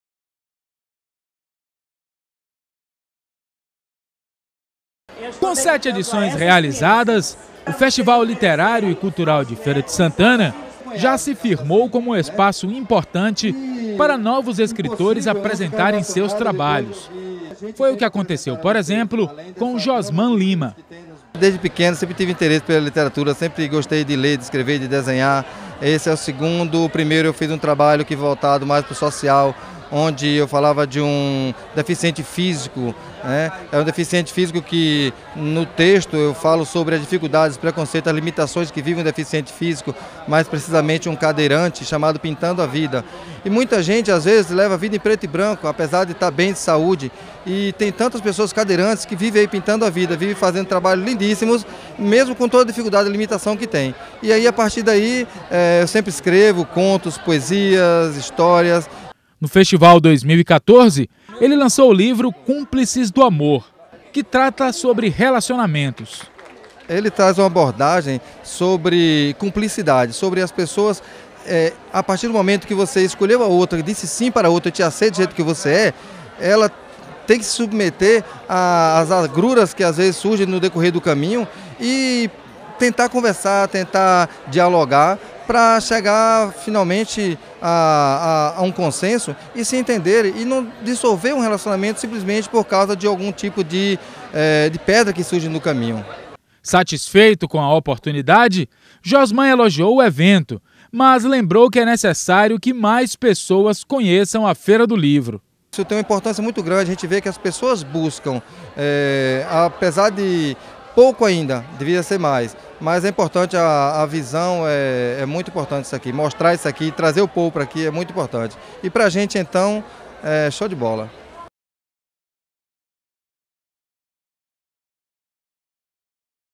Festival abre espaço para novos escritores
Os novos escritores têm no Festival Literário e Cultural de Feira de Santana um espaço cada vez mais consolidado para o lançamento de novos títulos, a exemplo do que ocorreu com Josman Lima, que apresentou o livro “Cúmplices do Amor”.

- Ficha Técnica / TV OLHOS D'ÁGUA -

Reportagem: Elsimar Pondé
Imagens: Jorge Menezes
Edição: Volney Menezes